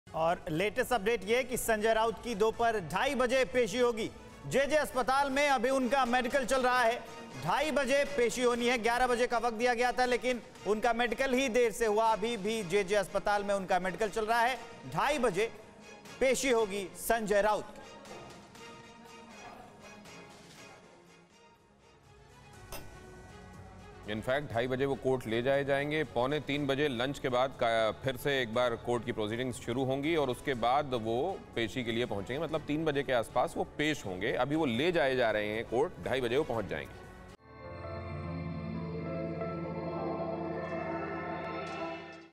Patra Chawl Land Scam: Sanjay Raut की दोपहर 2:30 बजे होगी पेशी
प्रवर्तन निदेशालय (ईडी) ने मुंबई की पात्रा चॉल घोटाले के मामले में रविवार को शिवसेना सांसद संजय राउत को गिरफ्तार कर लिया. अधिकारियों ने बताया कि राउत (60) को दक्षिण मुंबई के बलार्ड एस्टेट में ईडी के मंडल कार्यालय में छह घंटे से अधिक समय तक चली पूछताछ के बाद गिरफ्तार कर लिया गया. आज दोपहर 2:30 बजे संजय राउत की कोर्ट में पेशी होगी.
Credit